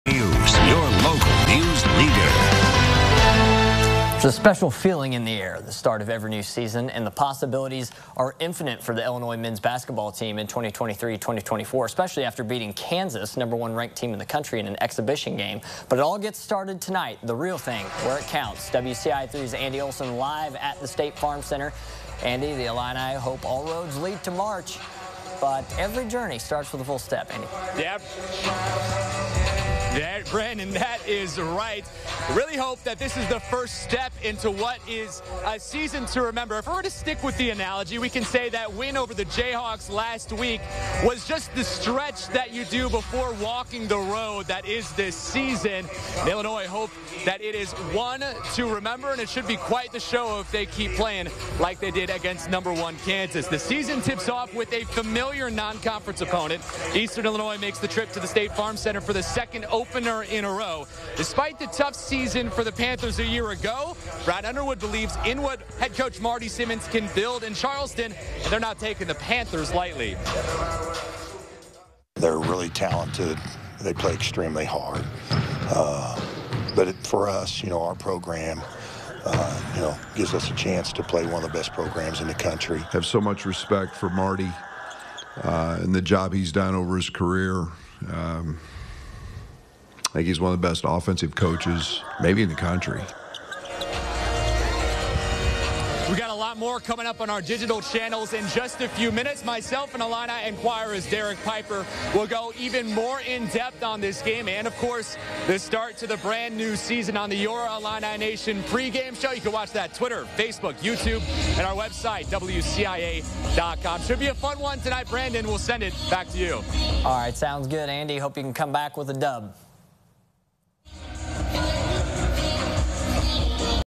LIVE! Illinois basketball tips off season hosting Eastern Illinois
CHAMPAIGN (WCIA) -- Illinois basketball tips off its season Monday night hosting Eastern Illinois at State Farm Center. The Illini open the season in the Associated Press Top 25 rankings for a program record fourth-straight season, coming in at No. 25. Meanwhile the Panthers are in year three of the Marty Simmons era, coming off a 9-22 season. These two teams also played in the season opener last November, with the Illini posting a 30-point win.